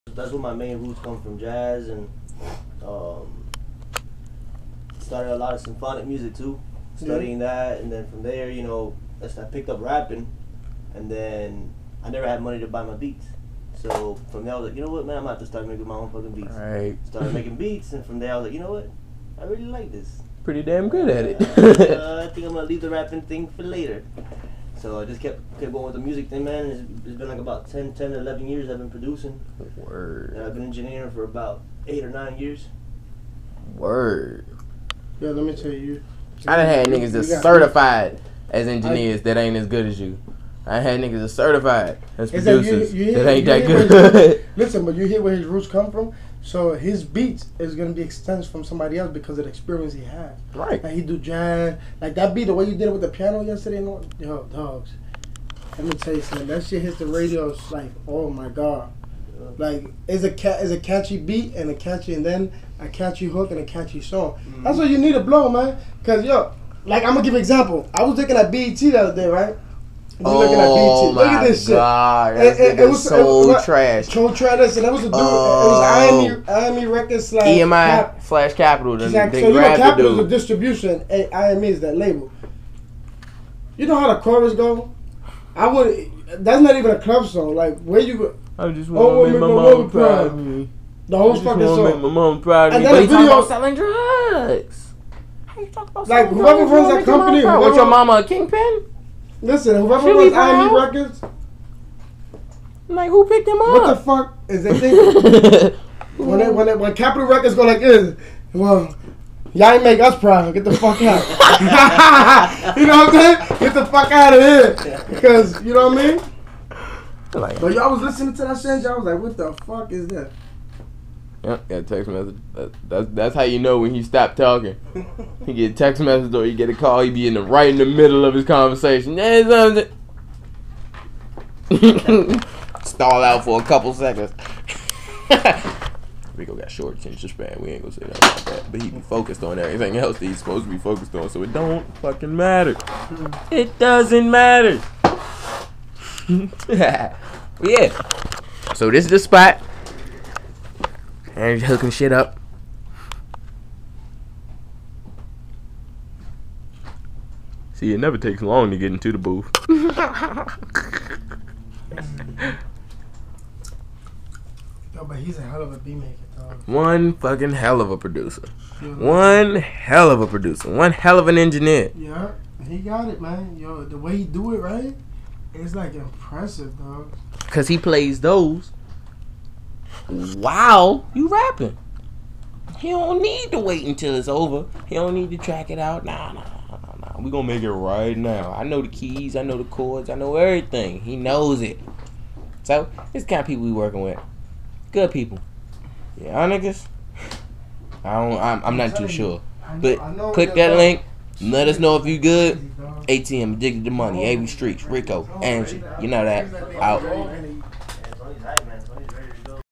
a.t.m Ent Avi Streetz Behind The Scenes W| Producer Engy & Manager Rico
behind the scenes A Day In The Studio With A.T.M Ent Avi Streetz